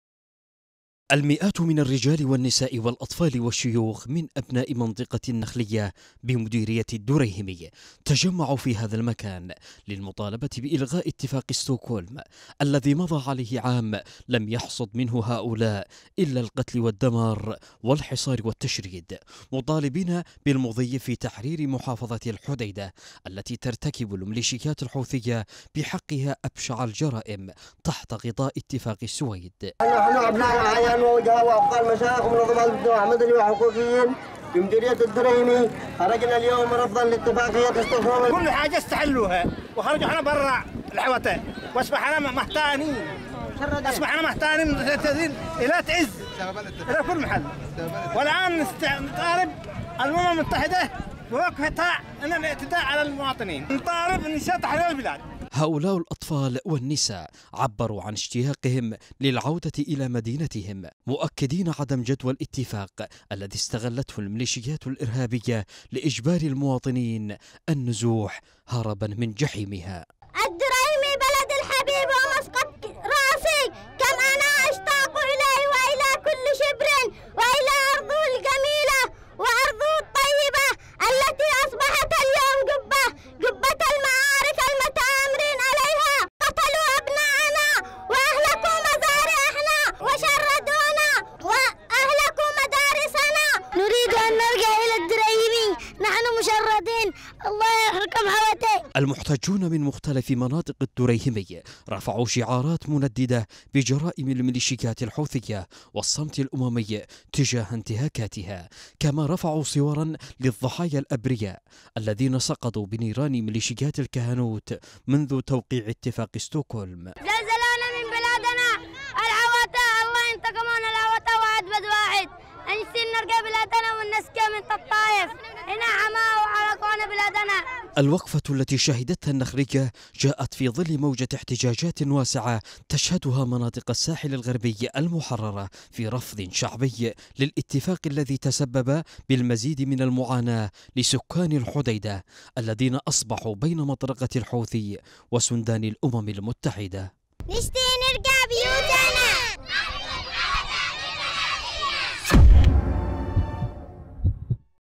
أصوات التهاميين ترتفع من النُخلية للمطالبة بإلغاء ستوكهولم - الحديدة
يوم تلو آخر ترتفع الأصوات المطالبة بإنهاء اتفاق  استكهولوم  الذي لم يجلب للحديدة سوى الموت والدمار وتحول من مشروع حل إلى مشكلة  مطالبة باستكمال تحرير الحديدة